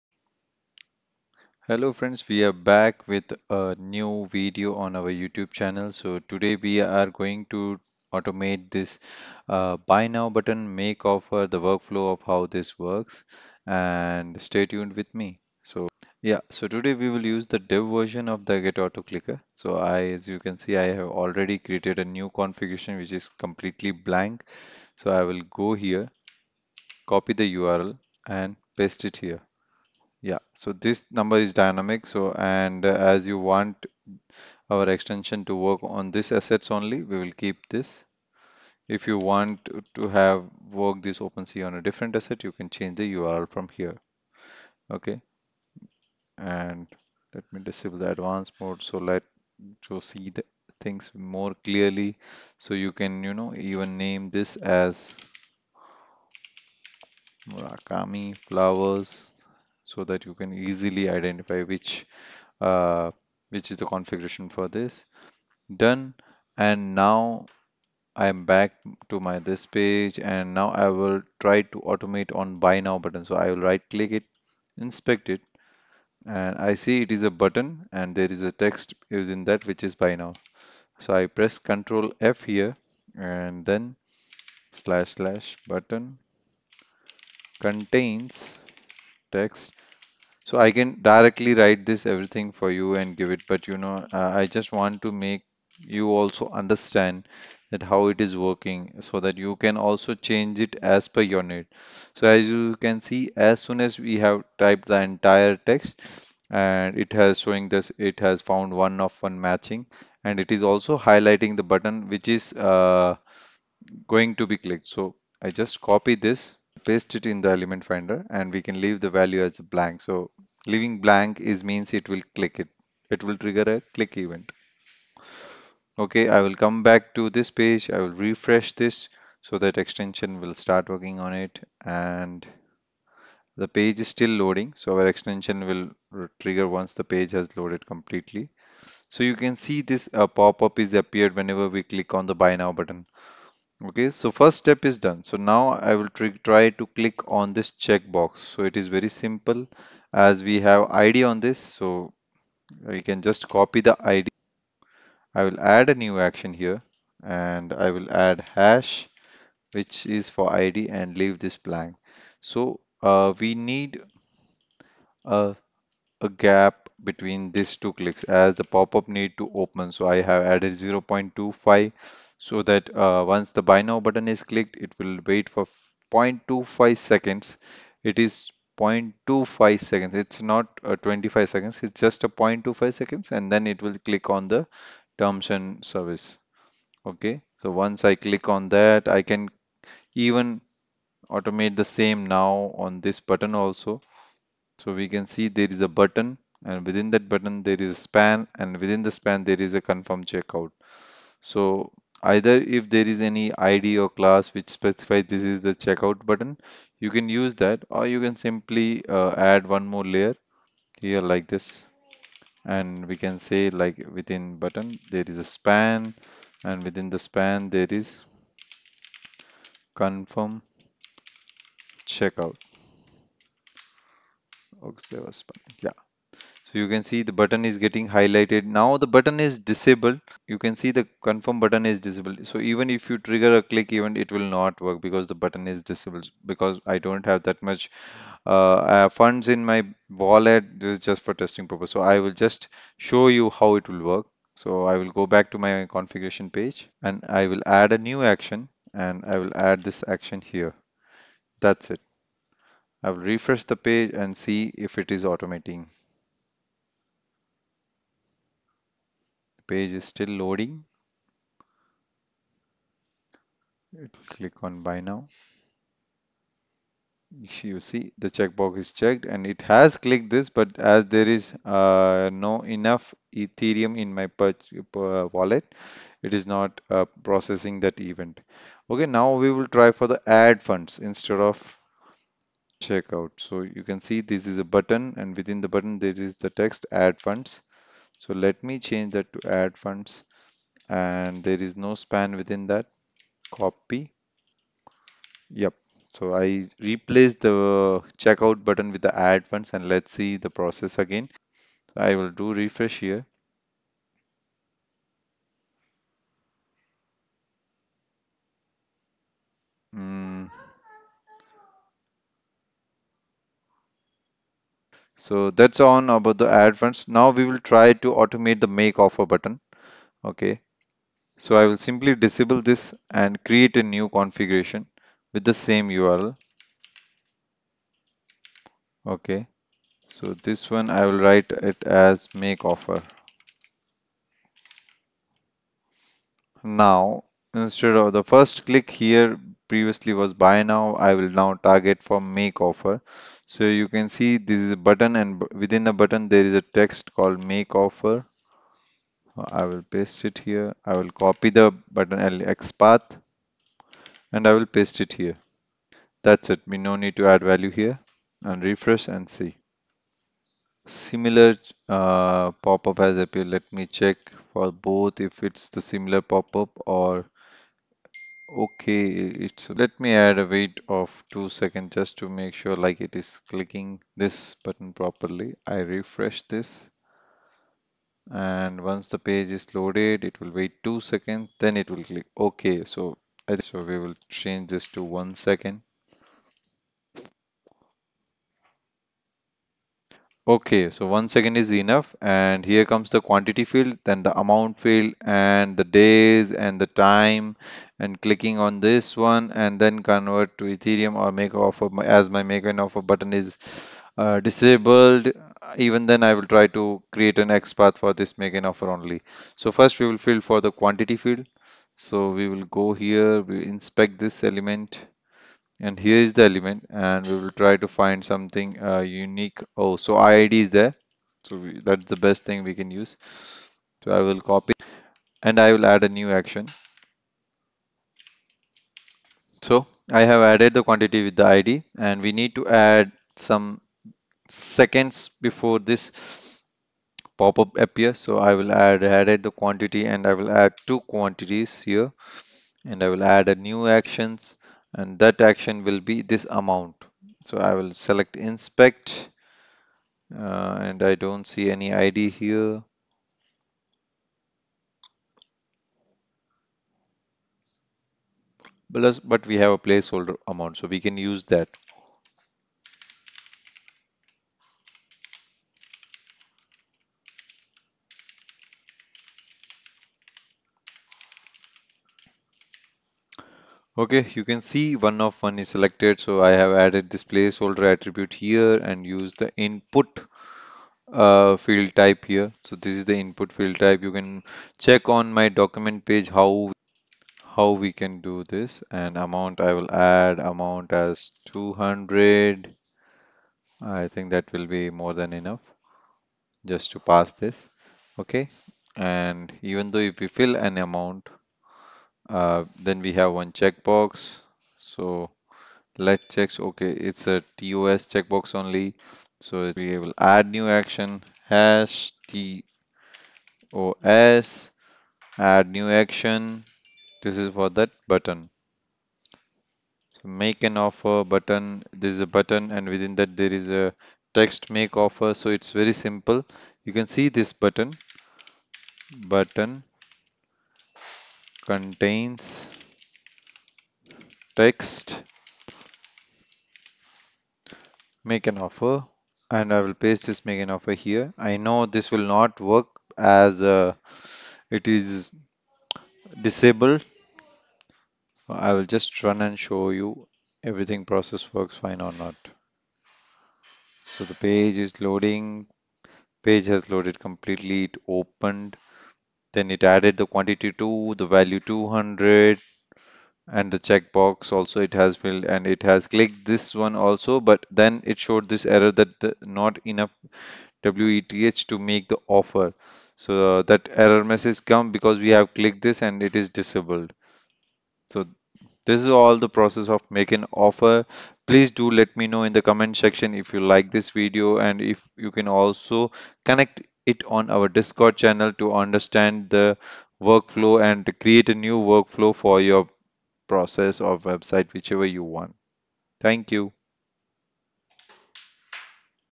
Auto Buy Now | Make Offer on NFT | Opensea auto place bid | with Auto Clicker Auto Fill
Now place bid on any of your Fav NFT using our Auto Clicker Auto Fill extension .

If you want you can map it with hotkey doing this you can perform all actions of configuration using Hotkey like Ctrl + P.

Fill Action and Click Action using Auto clicker.